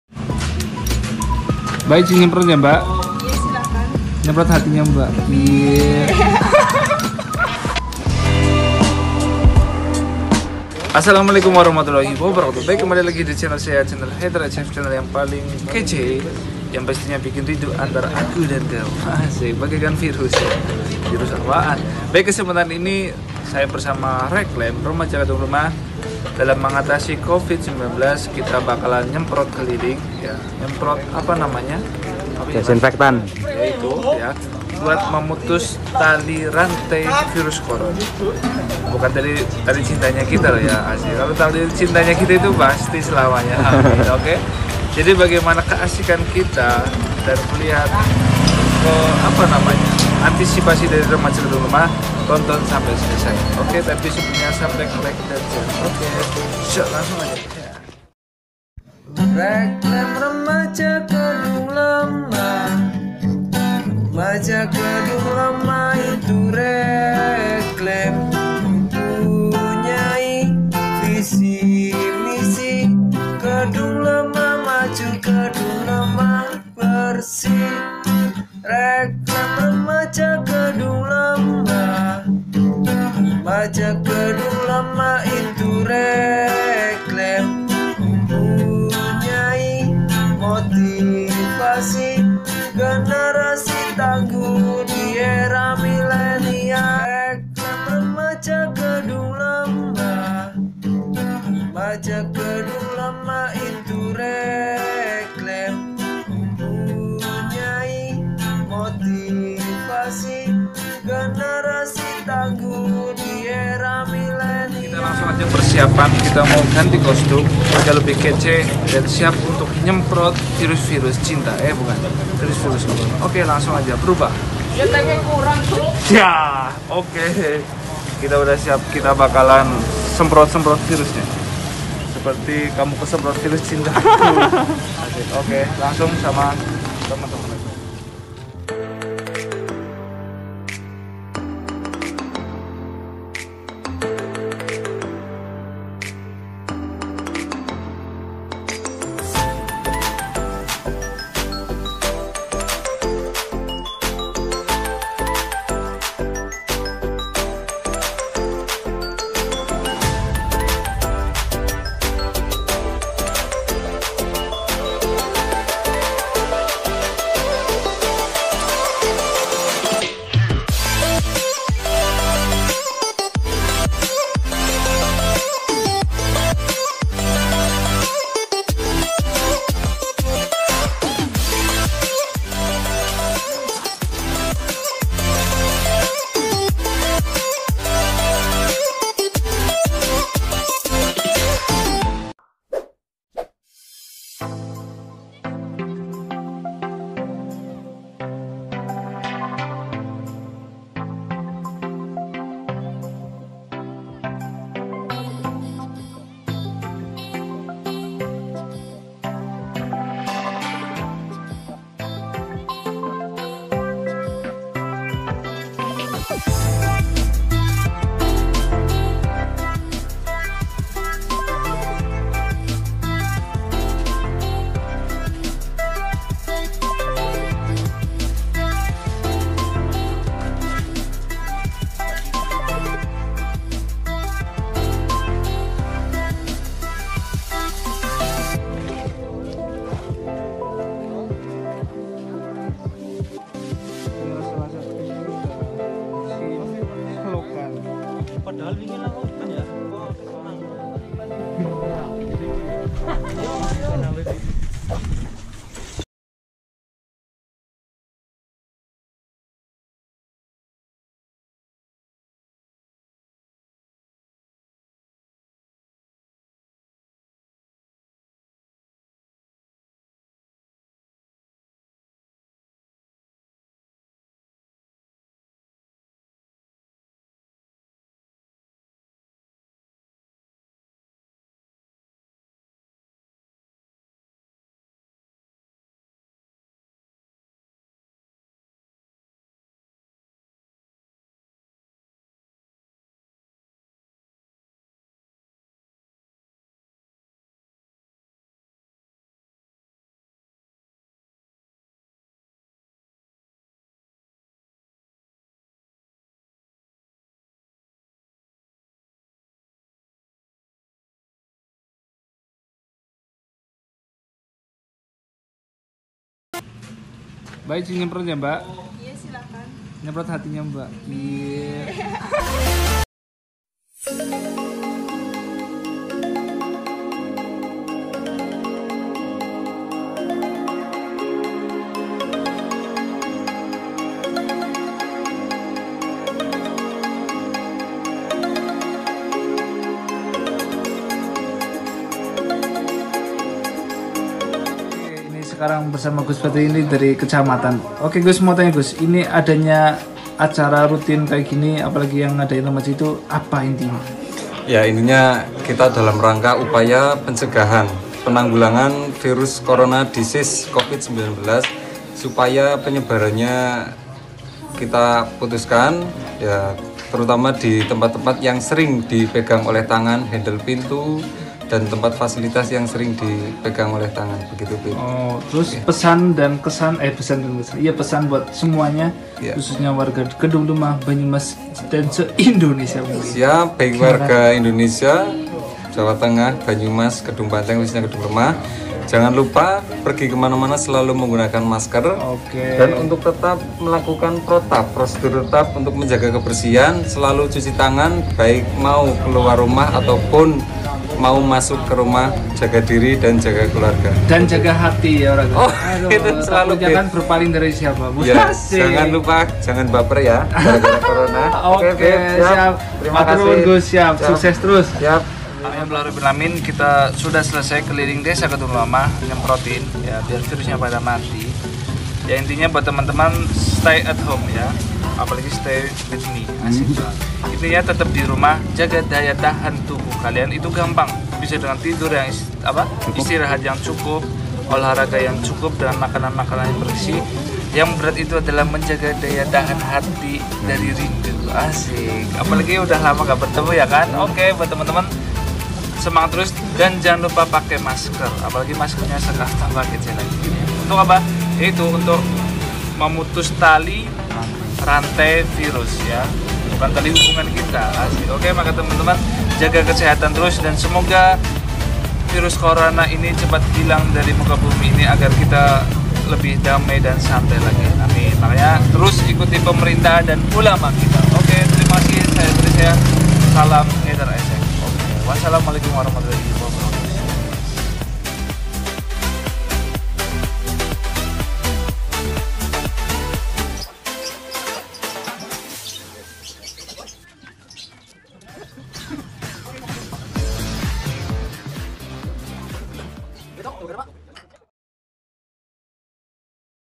Lawan VIRUS RINDU, eh. . Virus CORONA
Ini adalah salah satu kegiatan Sosial yang di lakukan oleh  REKLEM ( Remaja Kedunglemah ) di masa virus Corona ini, yang dimana kita berusaha untuk memutus tali rantai atau antisipasi adanya virus corona.

Kuy.. remaja yang lain smg bisa kompak kompak gini :)

Semoga Virus Corona ini segera hilang dan kita selalu diberi nikmat sehat dan panjang umur. aamiin

Drone : @ragilstudiochanel5667